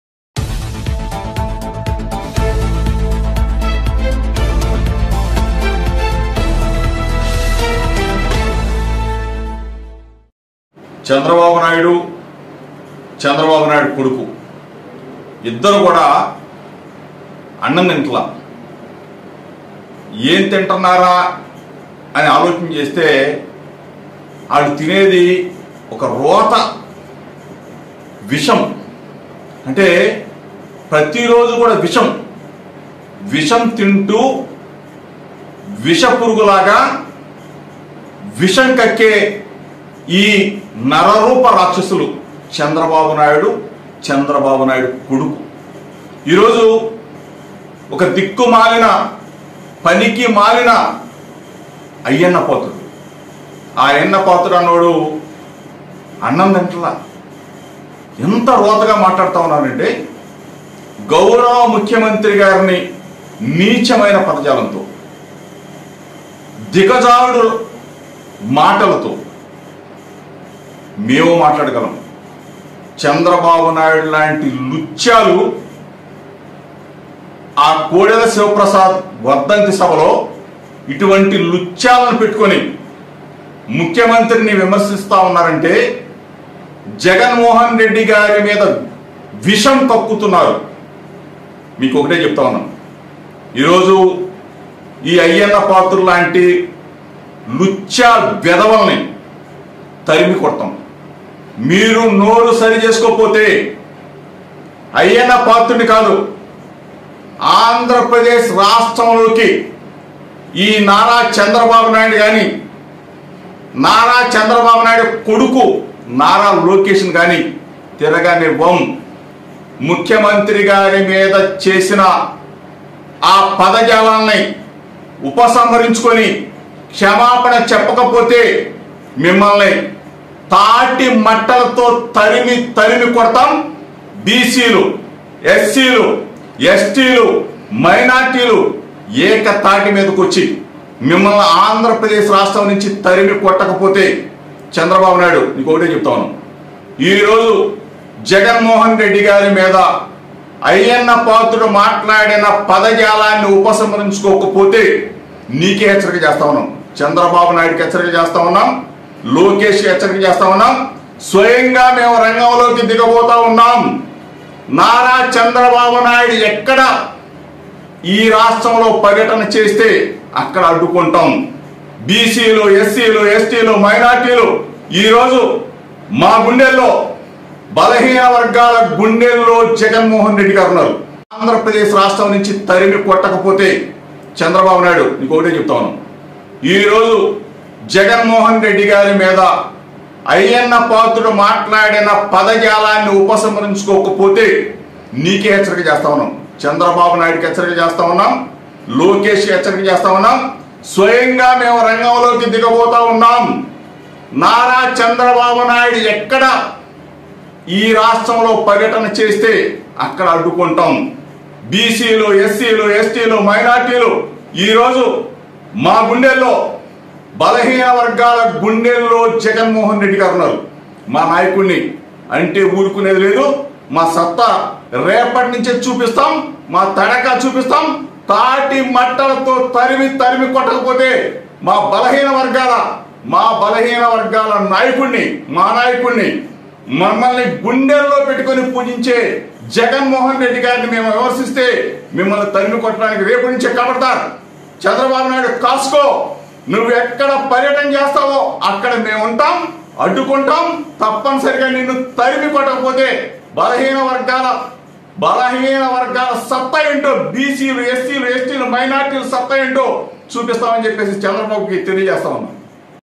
వాడొక లుచ్చా, కడుపుకు విషం తింటున్నారు, తాటి మట్టలతో తరిమి తరిమి కొడతా అంటున్న Ycp MLA Jogi Ramesh
కృష్ణా జిల్లా పెడన నియోజకవర్గం MLA Jogi రమేష్ ప్రెస్ మీట్